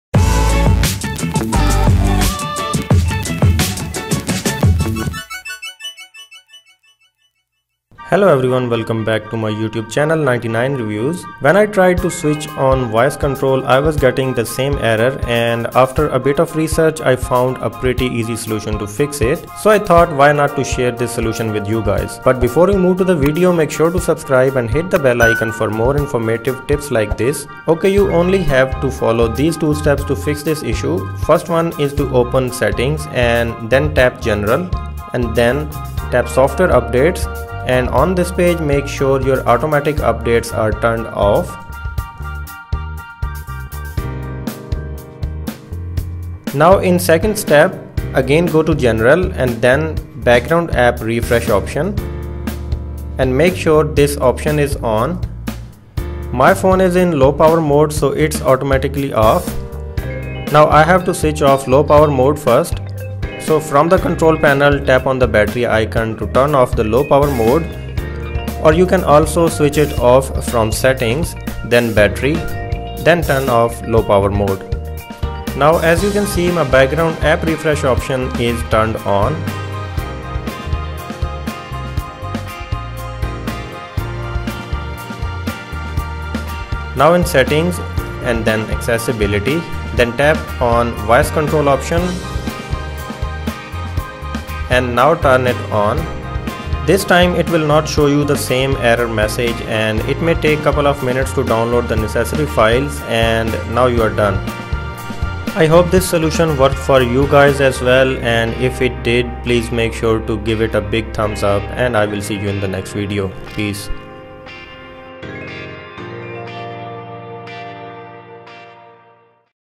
Quick Fix - Unable to turn on voice control error
Hello folks,
In today's video I will show you the solution of Unable to turn on voice control. Failed to download necessary files. Turn on voice control to try again error.
I have mentioned 2 steps and both are very simple, fiest step is to turn off automatic software updates and the second is to turn on background app refresh option.
After doing these 2 steps, turn your voice control on and it will take couple of minutes to download the necessary files and your voice control will be activated. This solution works for both iPhone and iPad.
I hope this solution works for you guys too.

===Social===

===Channel===